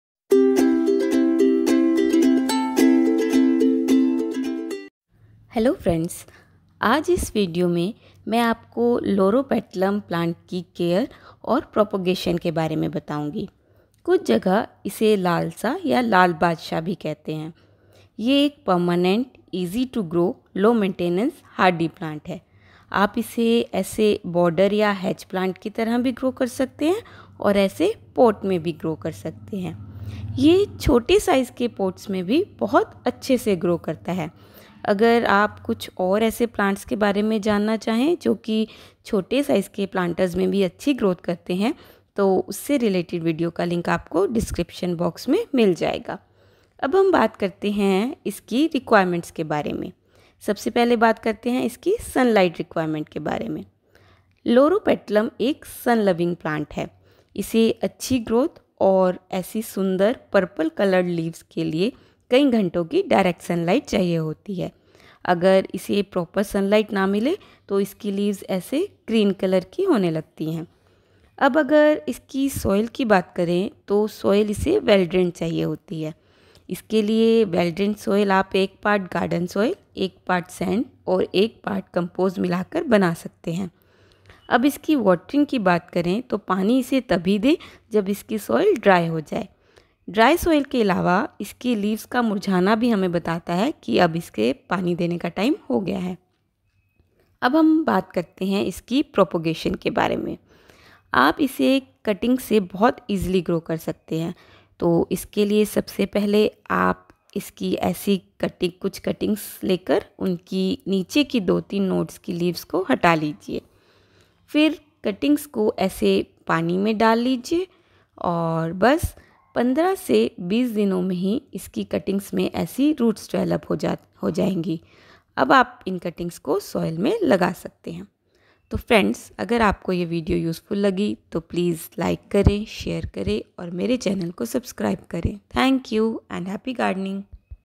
How to Grow & Care Loropetalum Plant/Red Leaf Plant Lalsa/Lal Badsha/Hedge Plant/Small Pot Plant
In this video I am telling about the care & propagation of Loropetalum plant/Lalsa/Lal badsha. It can be grown both as hedge or border plant and also in small sized pots also. It is a beautiful red or purple leaf plant...
All this I have learned from my personal practical experiences which include many failures also. I hope you find this video helpful...